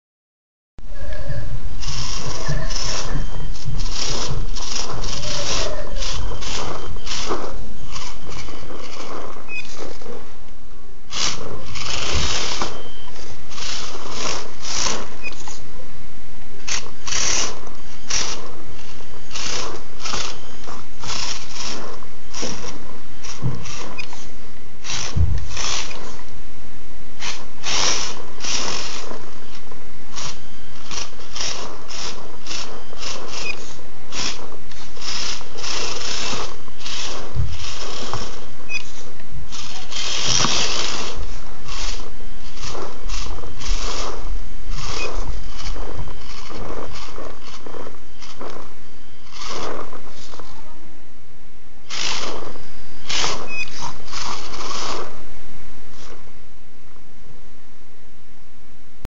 Andy's blade tmc first crawls
Losi micro blade chassis with torsionbar suspension